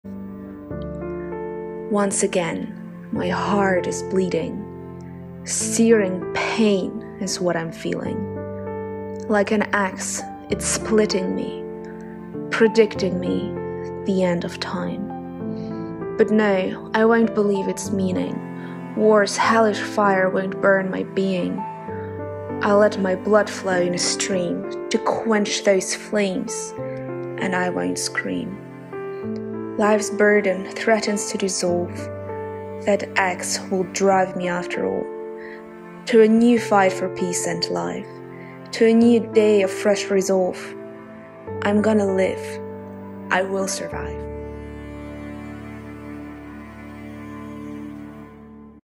Netherlands Voice for Artsakh. Armenia's soul is speaking... By Schago
A poem by Schago.
Translated from German into English by Mick Sobyanin and Schago.

Once again, my heart is bleeding
Searing pain is what I’m feeling
Like an axe, it's splitting me
Predicting me... the end of time...

But no, I won’t believe its meaning
War's hellish fire won’t burn my being
I’ll let my blood flow in a stream
To quench those flames... and I won’t scream!

Life's burden threatens to dissolve
That axe will drive me after all
To a new fight for peace and life
To a new day of fresh resolve
I'm gonna live, I will survive!

Speak up to save Armenian lives. Record this poem and publish on your pages.

On the morning of Sept. 27, 2020, Azeri forces began a large-scale attack of multiple fronts on Artsakh, also known as Nagorno-Karabakh. The decades-old fight of the people of Artsakh for self-determination has not been this bloody in 25 years.

Please, do not ignore this war! Stop the terrorism, stop the genocide threatening the Armenians.